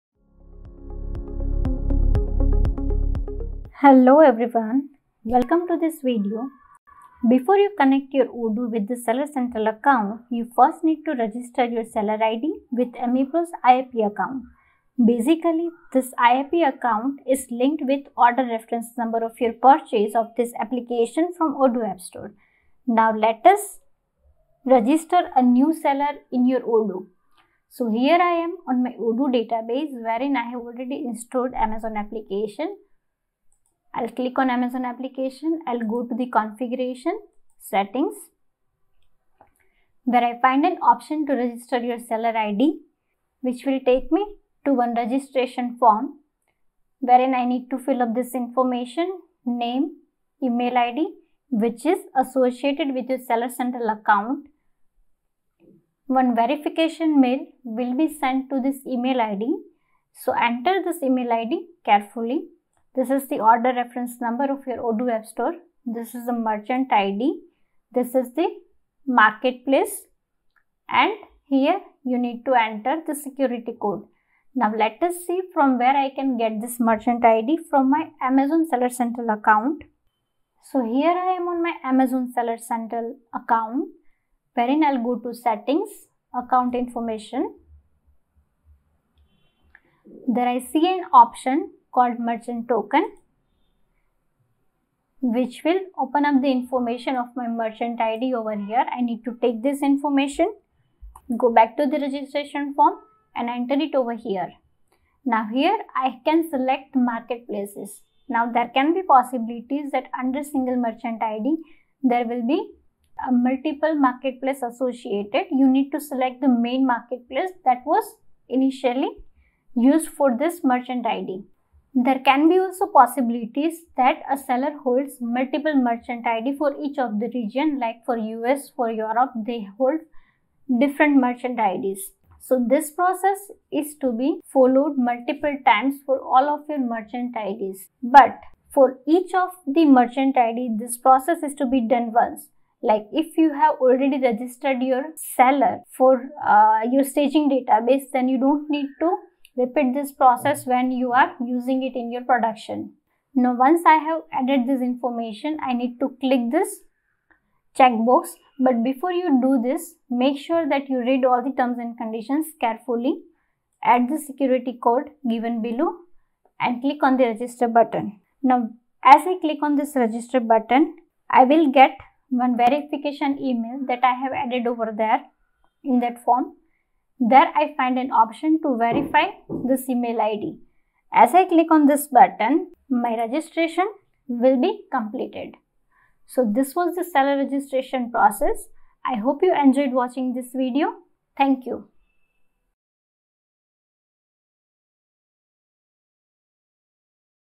Seller Registration Process - SP-API | Amazon Odoo Connector
Seller registration with Emipro is the first process that every seller would need to undergo in order to use Emipro Amazon Odoo Connector. This video will show you a detailed seller registration process.

Already purchased Amazon Odoo Connector and need support? Connect with Emipro Help Desk from the below link: